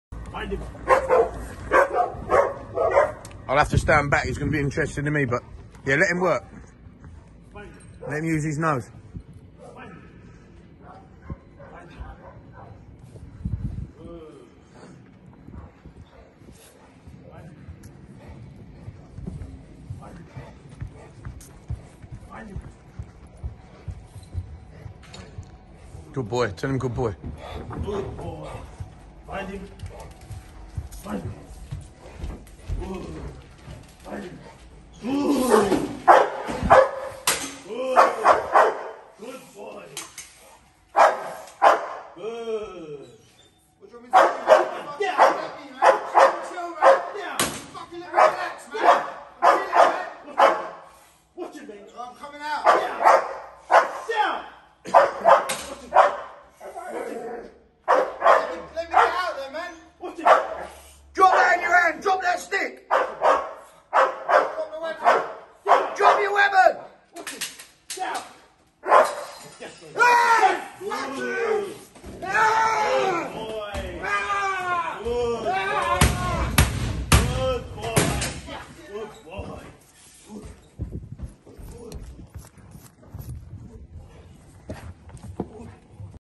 Working dog Bane
OTAC's bulldog Bane learning to use his nose in searching  for a criminal for bite work scenario.